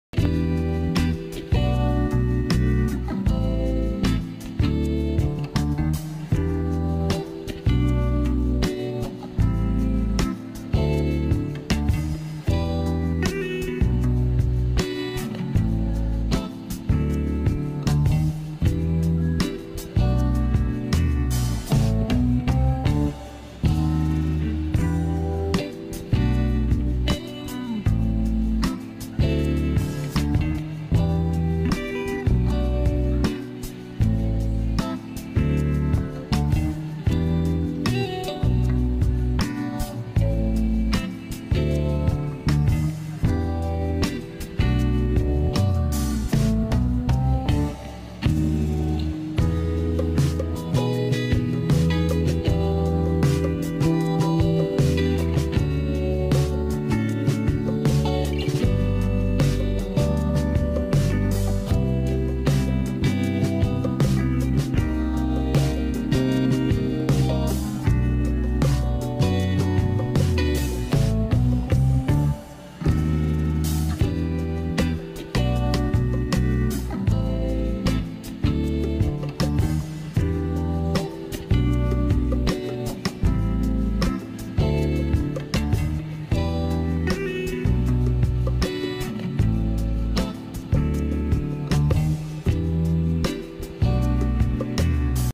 Pekingese Siblings' Love - I'll always by your side
Girl is Galina & boy is Gordon.